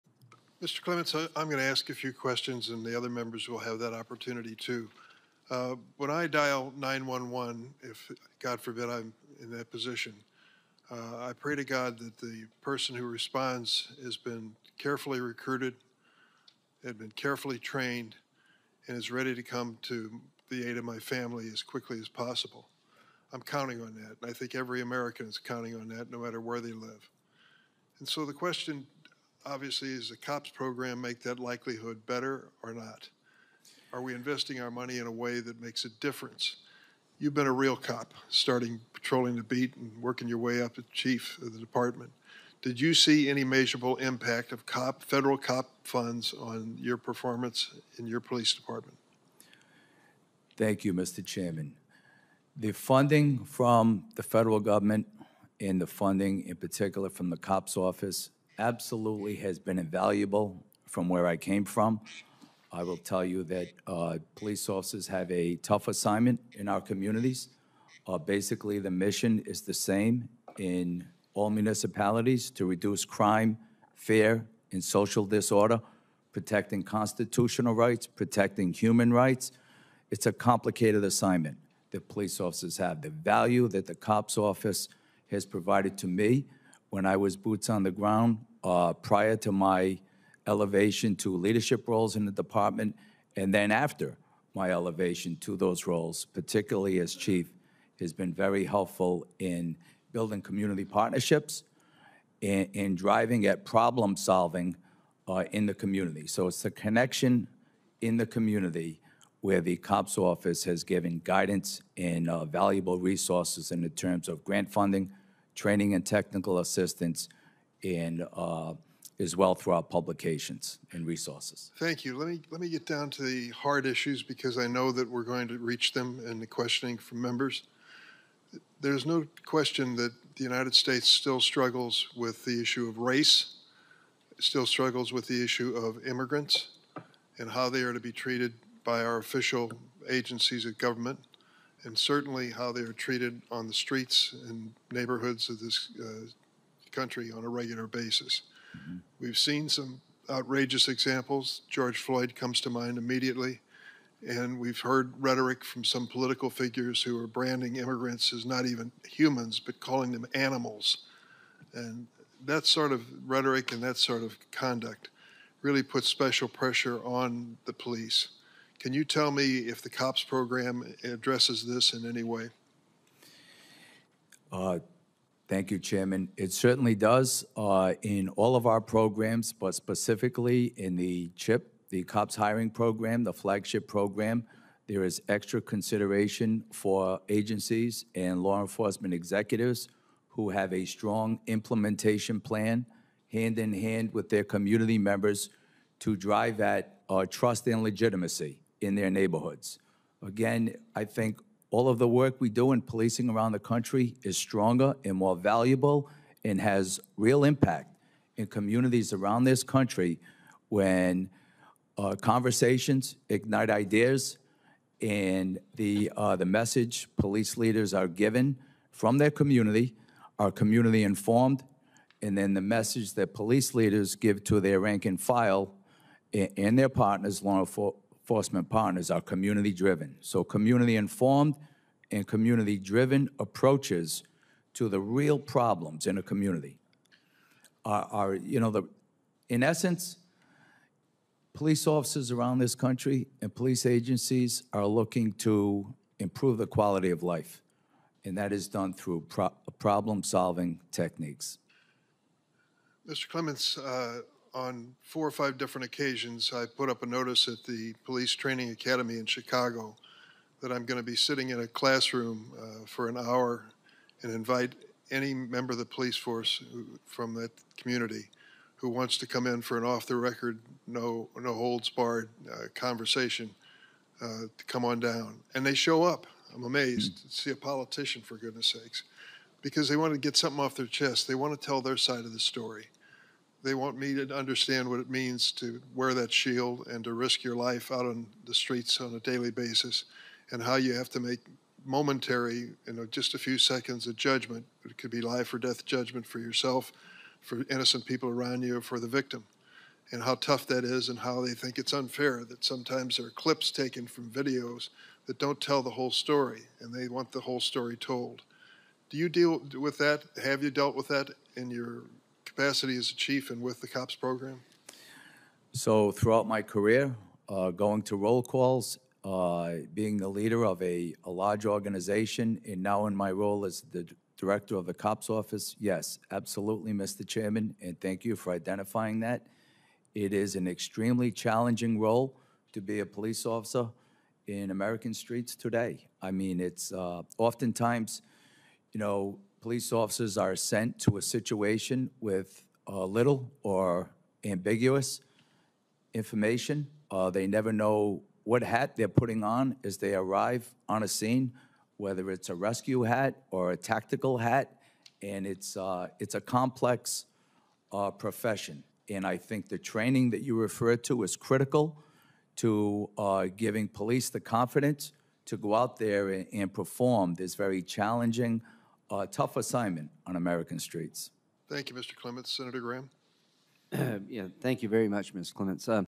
Dick Durbin Presses Witness On Whether COPS Program Addresses Police Brutality & Harmful Rhetoric
During a Senate Judiciary Committee hearing prior to the Congressional recess, Sen. Dick Durbin (D-IL) questioned witnesses about the COPS Grant Program and police brutality and harmful rhetoric.